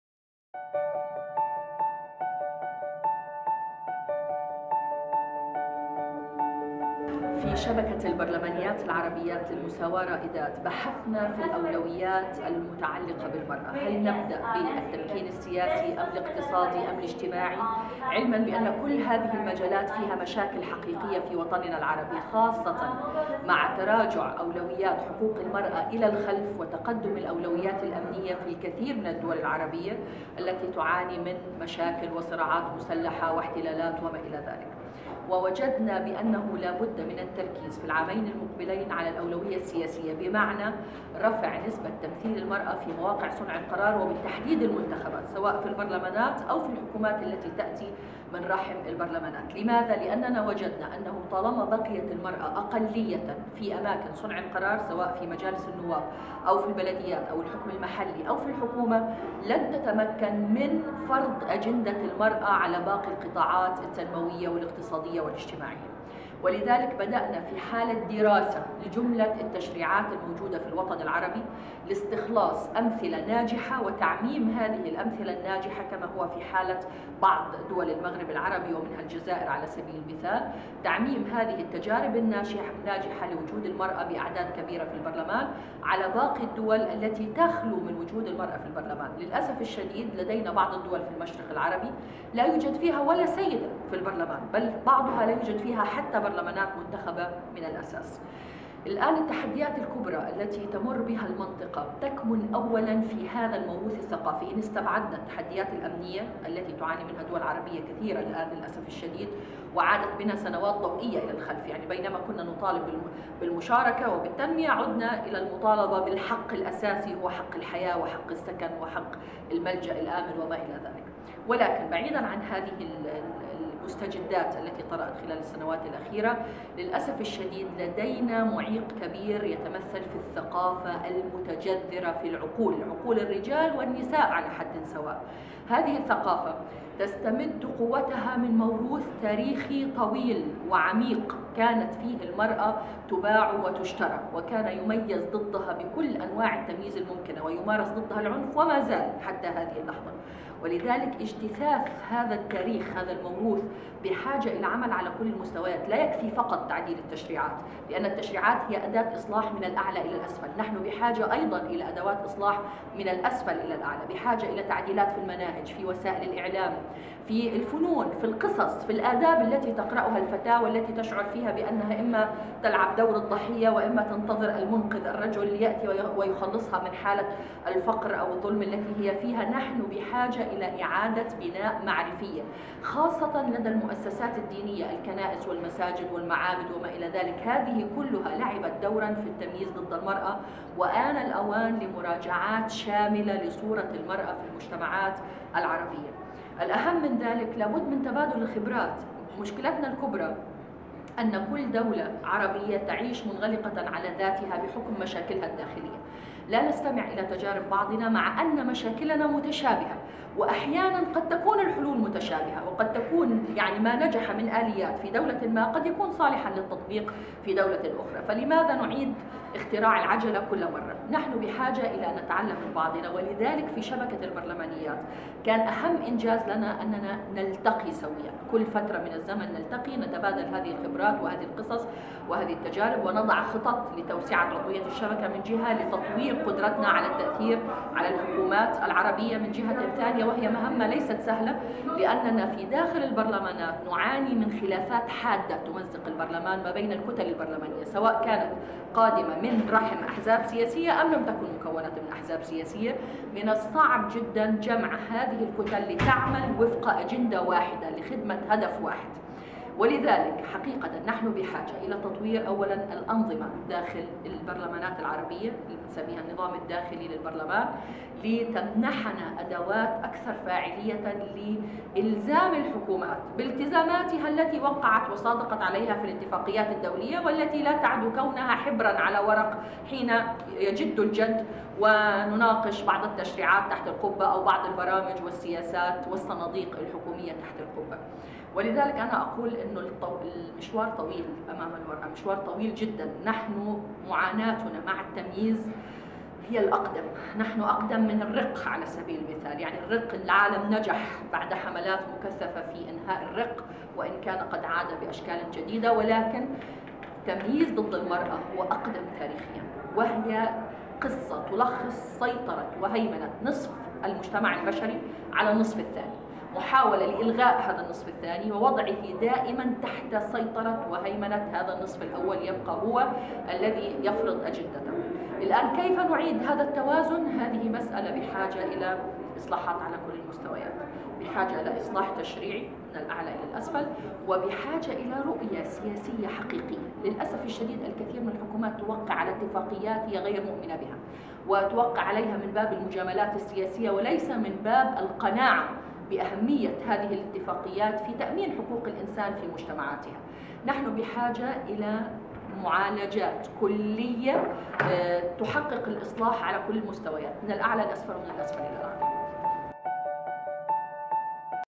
iKNOW Politics Interview with Rula Alhroob (Arabic)
Rula Alhroob is a Member of Parliament in Jordan. She is also the President of the newly formed Arab Women Parliamentarians Network for Equality "Ra'edat". This interview was conducted during the Inter-parliamentary Union annual parliamentary day meeting, in cooperation with UN Women on March 15, on the sidelines of the CSW 60.